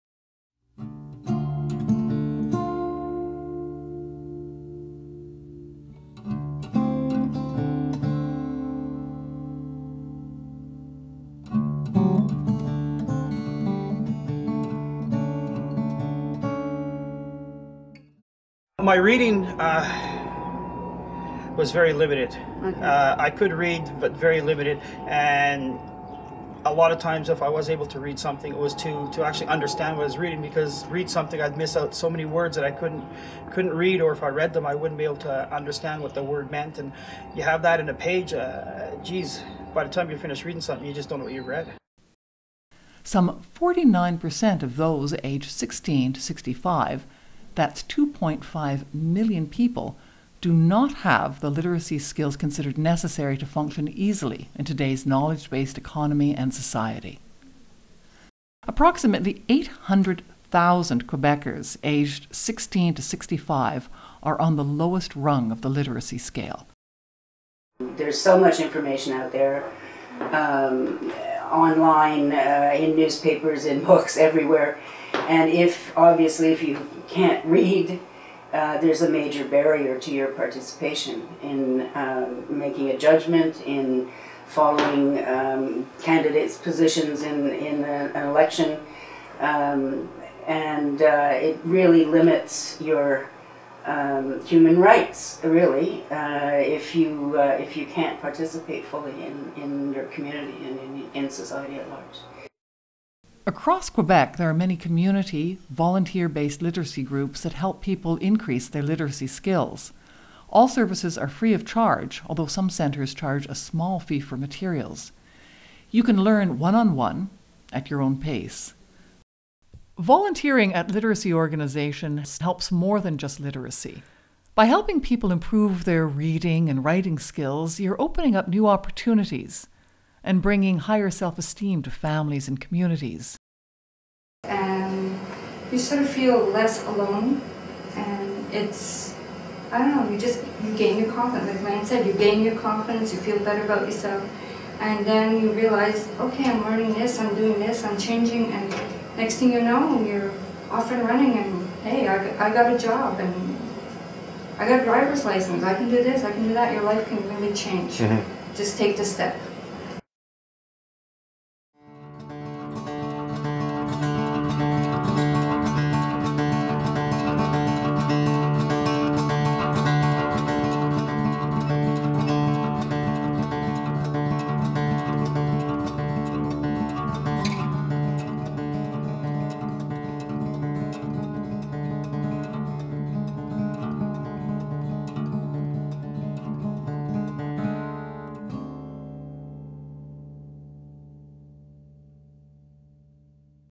Literacy - just take the step
Just take the first step to improve your reading: contact Literacy Volunteers of Quebec.  We can help you.

LVQ, building literacy one reader at a time.